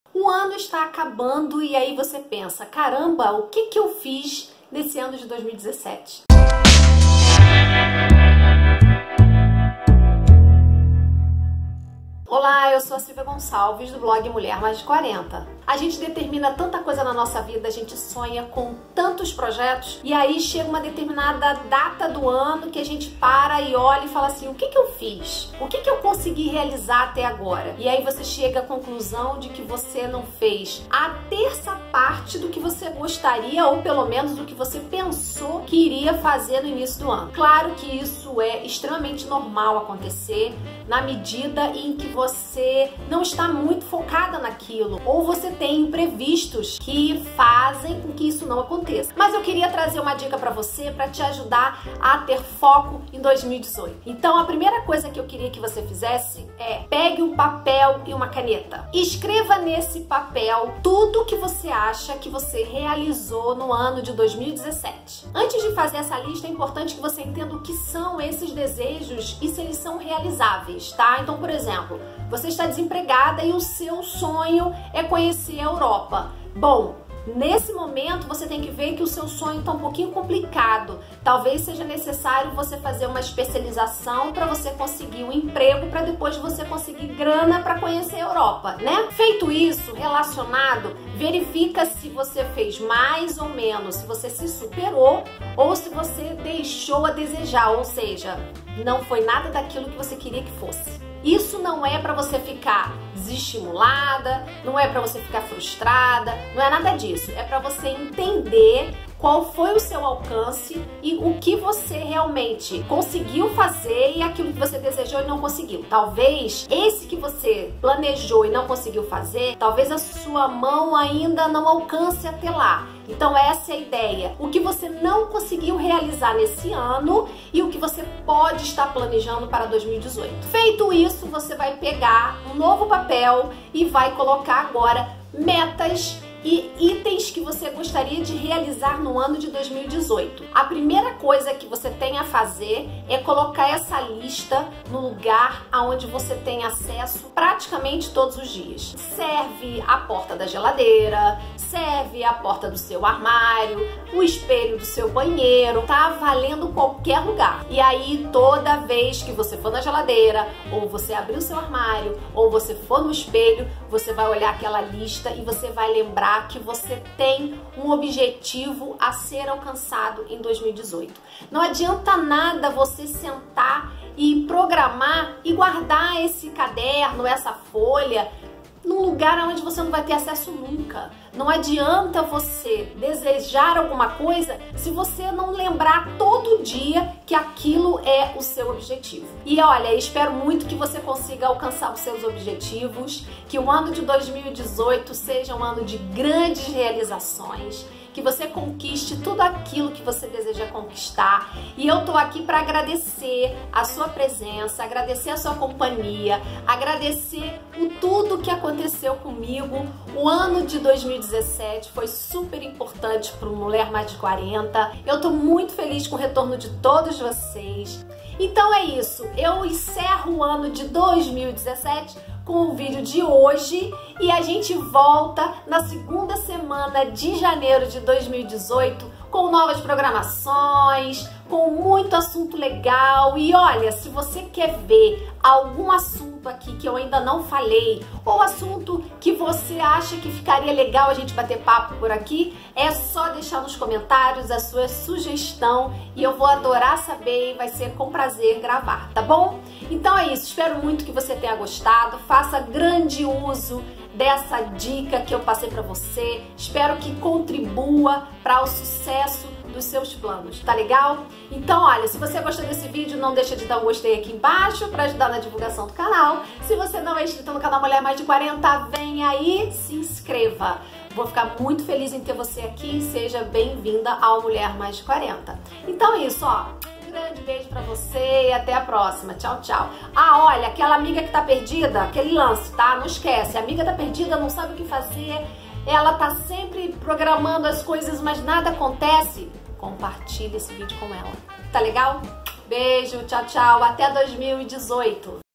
JÁ PLANEJOU SUAS METAS PARA 2018? - MULHERMAISDE40
Já planejou suas metas para 2018? Se em 2017 você não conseguiu realizar tudo que planejou, confira essa dica simples e eficaz! Que seu ano de 2018 seja produtivo, que você alcance suas metas e objetivos planejando tudo que quer realizar.

Silvia Gonçalves